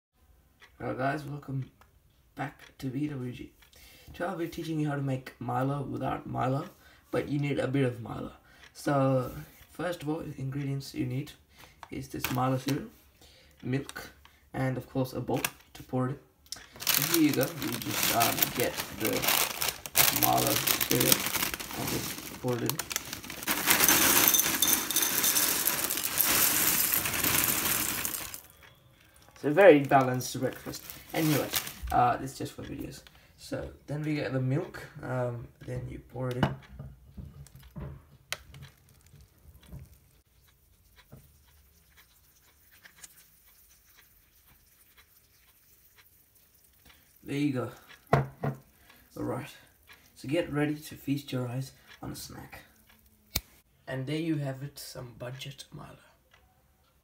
How To Make Budget Australian Milo Without Milo (360p Meme Quality)
How to make Milo without Milo but you need a bit of Milo